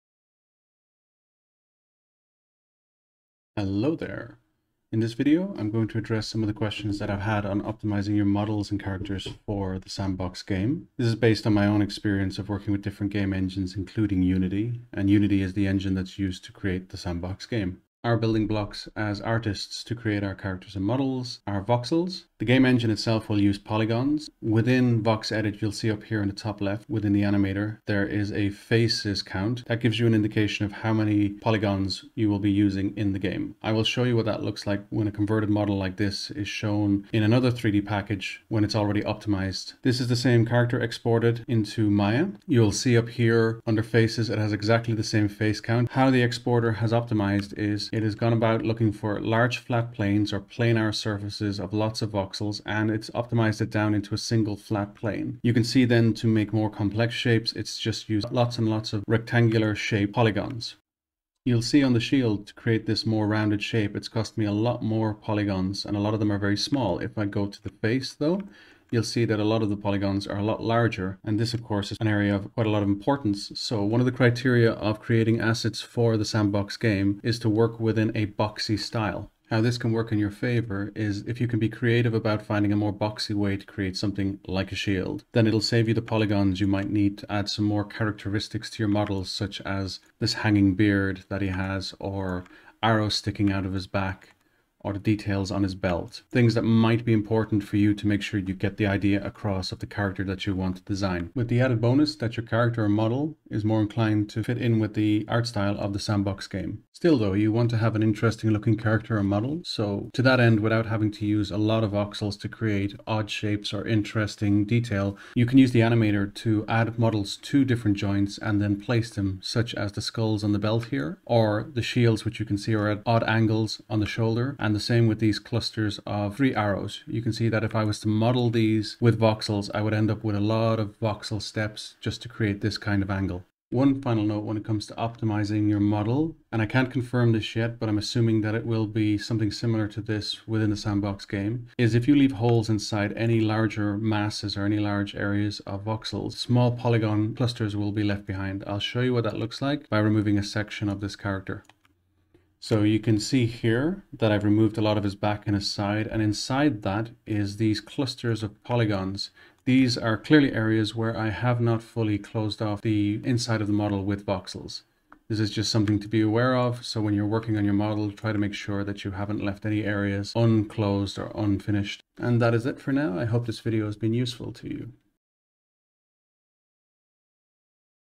HOW TO Optimize your voxels for the SandBox game and VOXEDIT
I apologise for the poor audio quality in this video.
This video is to answer questions about how to make sure your characters and models meet the face count limitations to be accepted into the Sandbox game.